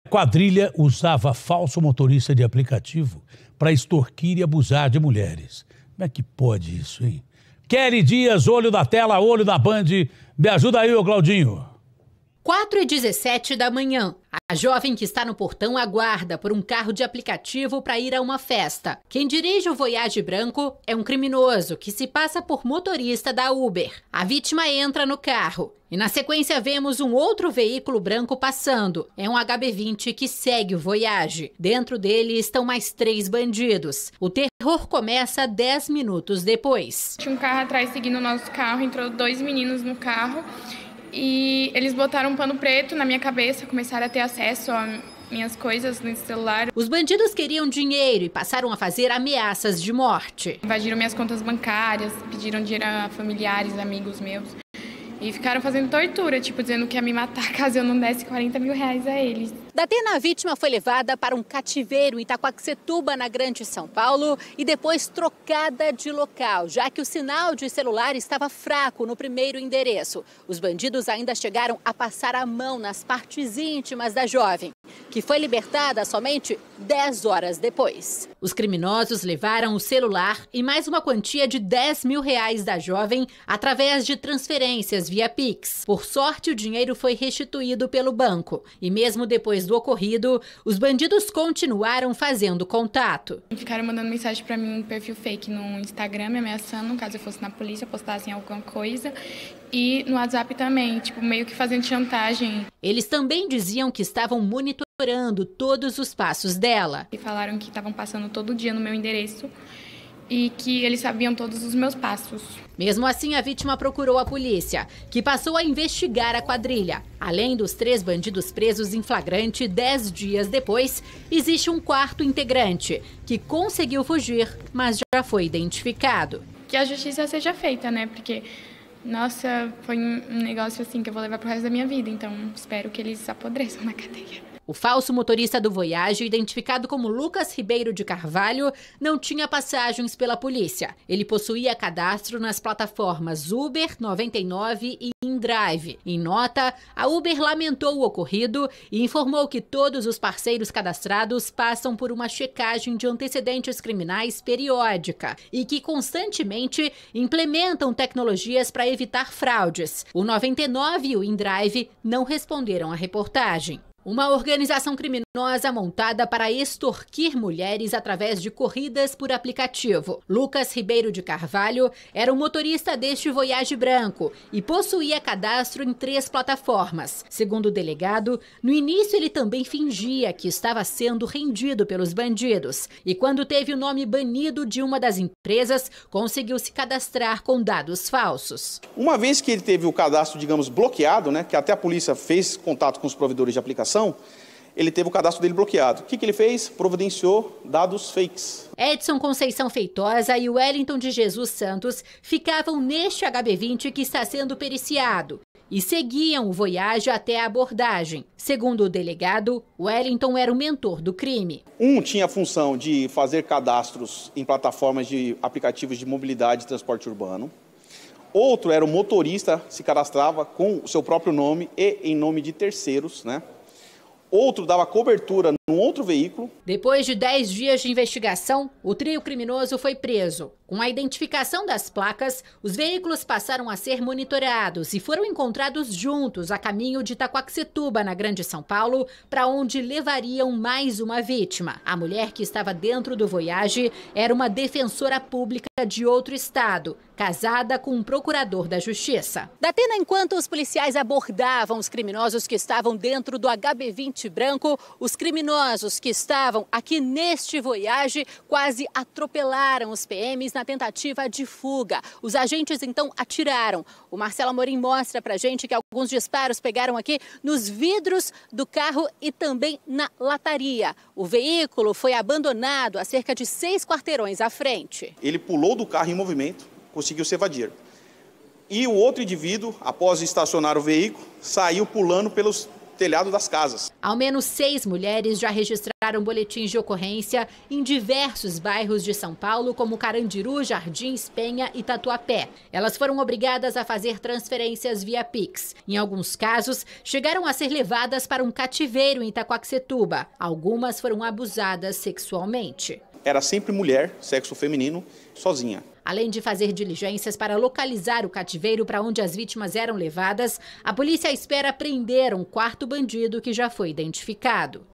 Falso motorista: quadrilha extorquia e abusava de mulheres
Bandidos fingiam ser motoristas de aplicativo para sequestrar, extorquir e abusar de mulheres.